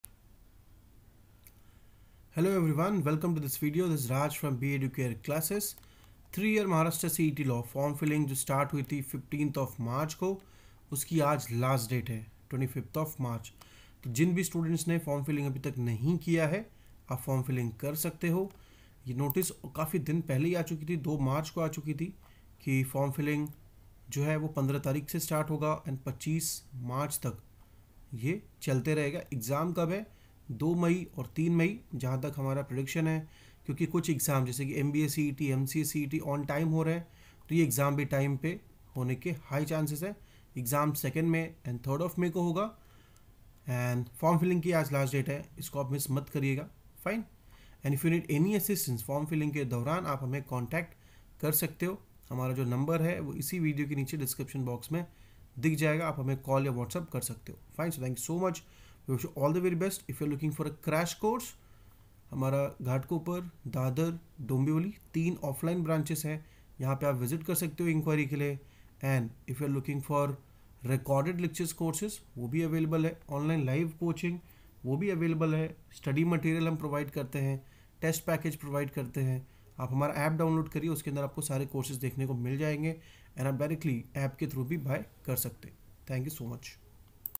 Today is Last Day to Fill LAW CET Form - 3 Year MH LAW CET 2023 | Video
Today is Last Day to Fill CET Form - 3 Year MH LAW CET 2023 | Video by  @BEduCAREclasses

3yearllb
5yearllb

Join Classroom Coaching / Online LIVE Lectures / Video Lectures courses / Books / eBooks / Test Packets  & Start your Preparation -